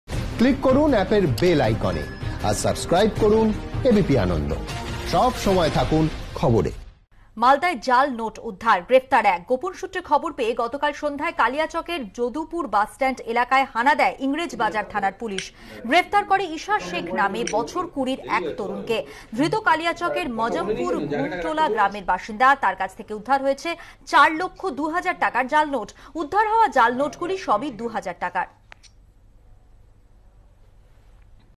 মালদায় জাল নোট উদ্ধার, গ্রেফতার এক| ABP Ananda
ফের মালদায় জাল নোট উদ্ধার। গ্রেফতার এক। গোপন সূত্রে খবর পেয়ে গতকাল সন্ধেয় কালিয়াচকের যদুপুর বাস স্ট্যান্ড এলাকায় হানা দেয় ইংরেজবাজার থানার পুলিশ। গ্রেফতার করে ইশা শেখ নামে বছর কুড়ির এক তরুণকে। ধৃত কালিয়াচকের মজমপুর ঘুনটোলা গ্রামের বাসিন্দা। তার কাছ থেকে উদ্ধার হয়েছে ৪ লক্ষ ২ হাজার টাকার জাল নোট। উদ্ধার হওয়া জাল নোটগুলি সবই ২ হাজার টাকার।